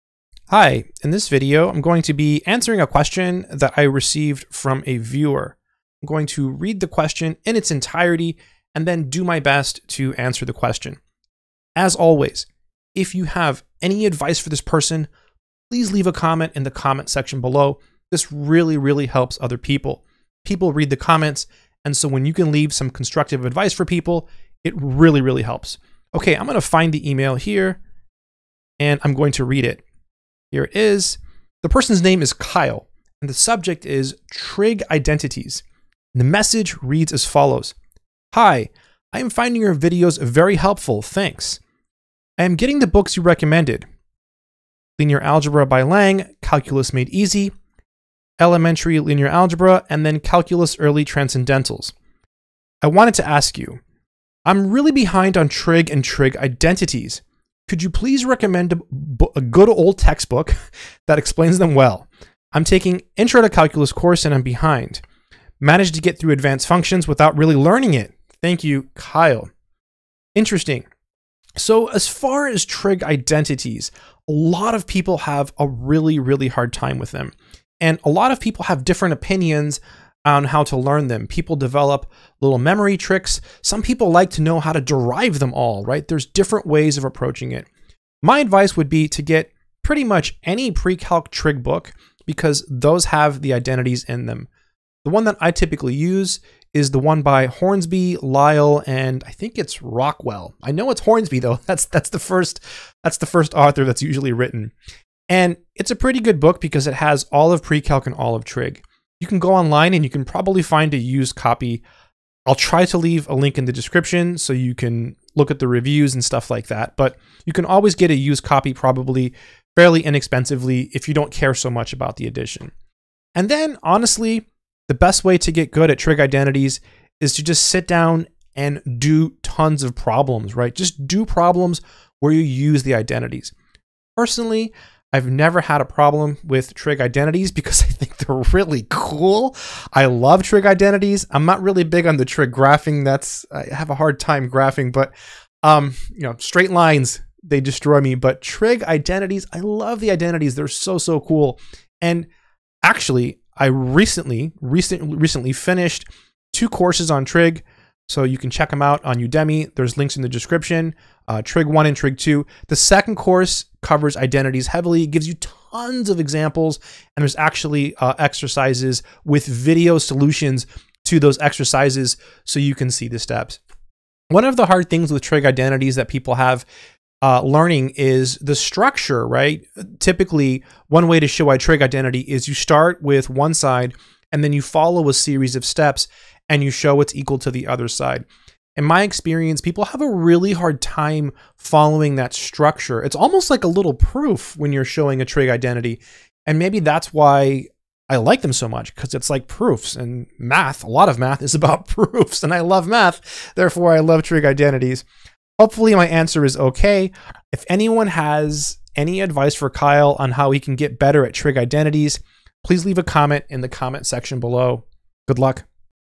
The Best Way to Master Trigonometric Identities
In this video I answer a question I received from a viewer. He is asking about learning trigonometric identities. Do you have any advice? If so, please leave a comment in the comment section below.

The book I mentioned in the video